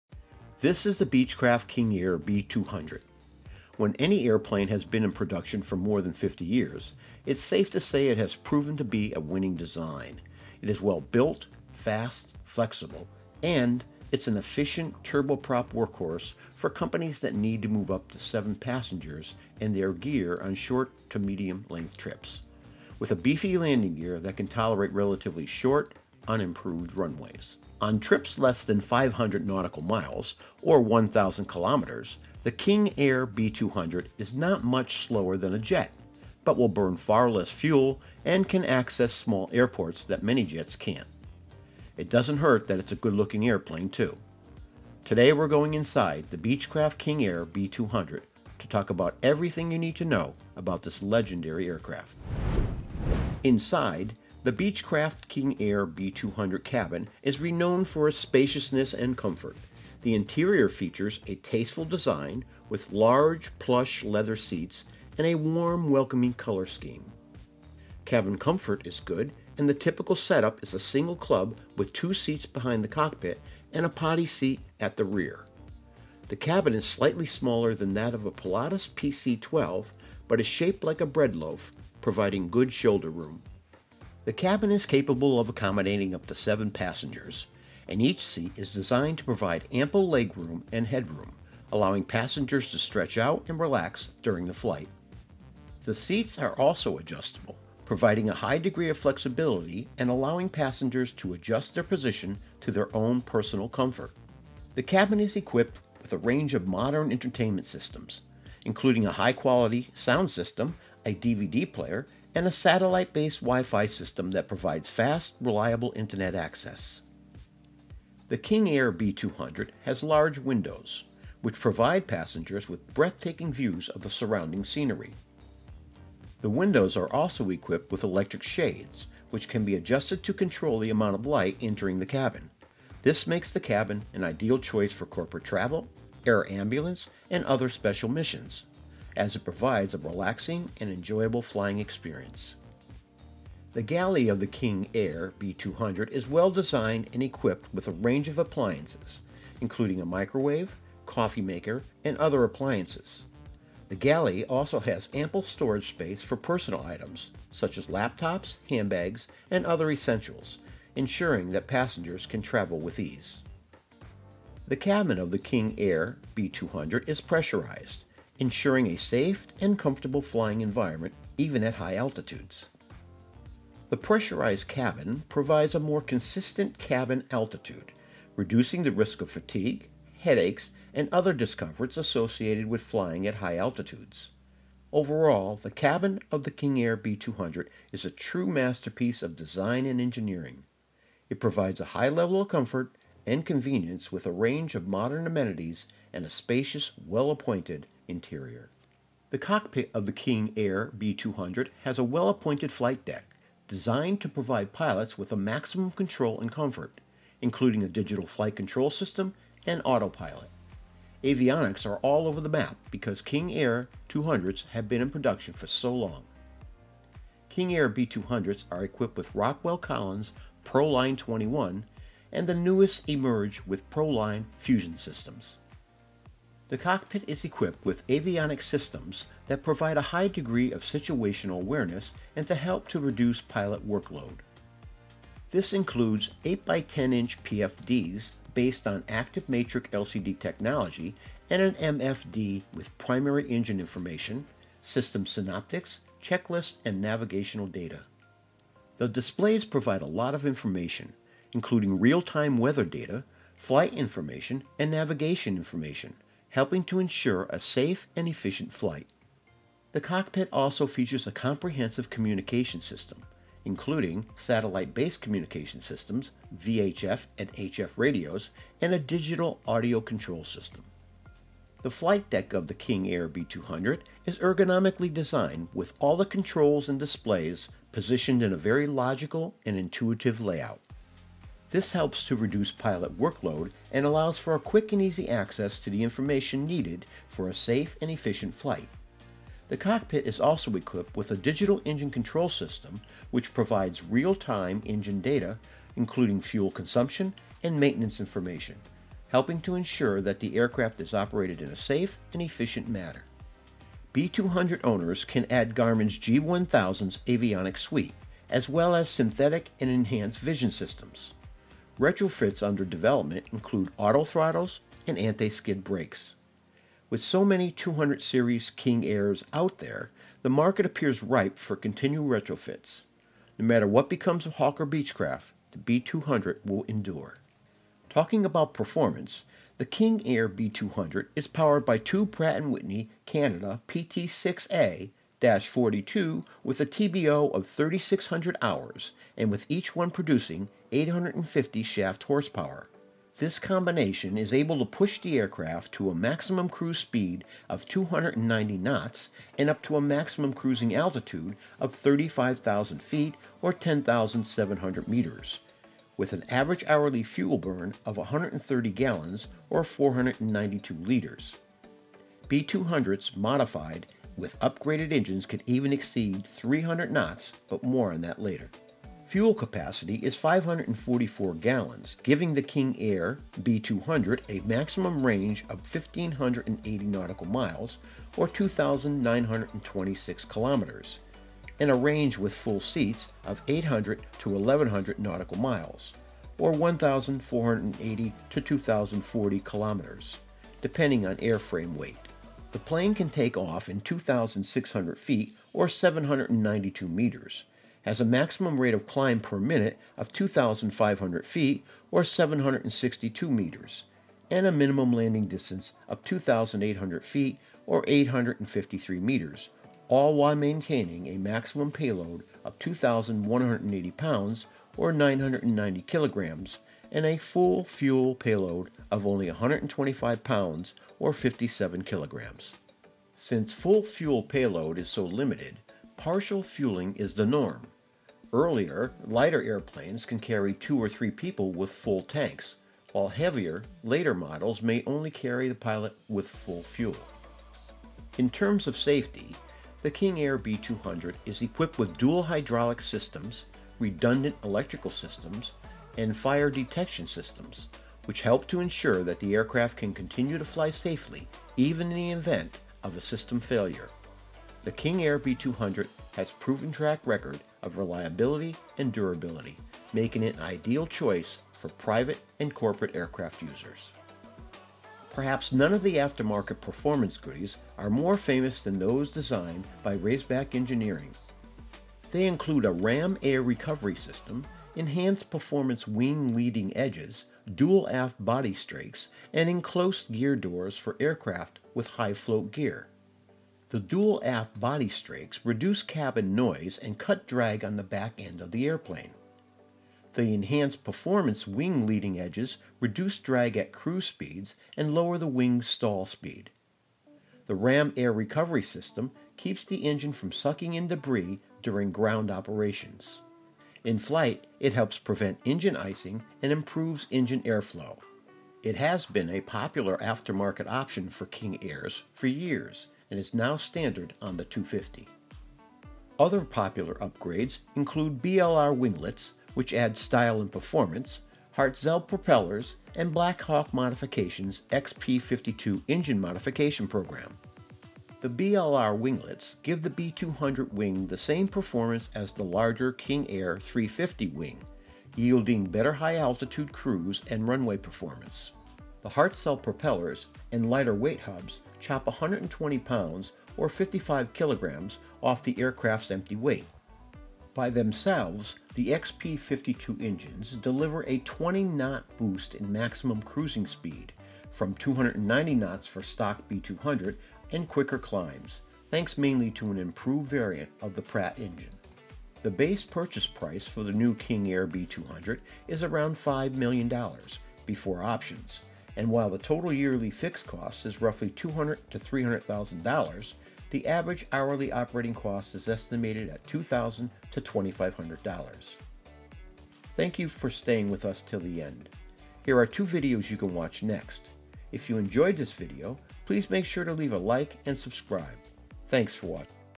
Inside The $5 Million Beechcraft King Air 200
Inside The $5 Million Beechcraft King Air 200 Plane

This is the Beechcraft King Air B200. When any airplane has been in production for more than 50 years, it’s safe to say it has proved to be a winning design. It is a well-built, fast, flexible, efficient turboprop workhorse for companies that need to move up to seven passengers and their gear on short to medium-length trips, with a beefy landing gear that can tolerate relatively short, unimproved runways.
On trips less than 500 nautical miles or 1,000 kilometers, the King Air B200 is not much slower than a jet, but will burn far less fuel, and can access small airports that many jets can’t. It doesn’t hurt that it’s a good-looking airplane, too.
The base purchase price for a new King Air B200 is around $5 million before options, and while the total yearly fixed cost is roughly $200,000 to $300,000, the average hourly operating cost is estimated at $2,000 to $2,500.
Here is everything you need to know about the Beechcraft King Air 200.

TIMESTAMPS
0:00 - Intro
0:52 - Cabin
3:18 - Cockpit
5:27 - Performance
7:44 - Popular Mods
9:22 - Prices
9:40 - Outro